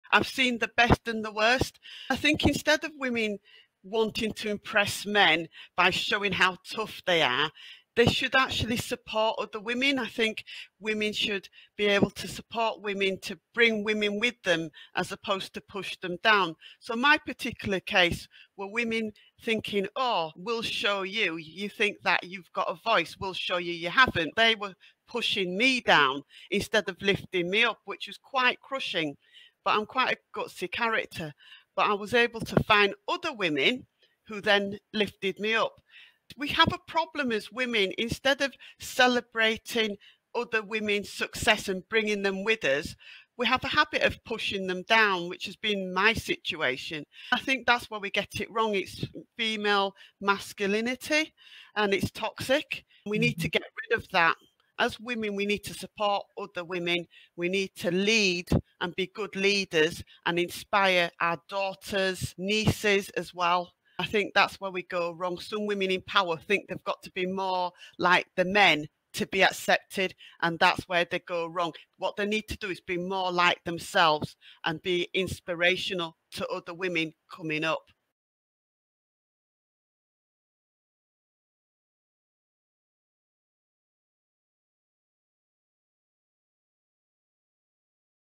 Women Need To Support Other Women
There are some workplaces where women can be their own worst enemy. We should always lift each other up, not tear each other down, says Dawn Maria France.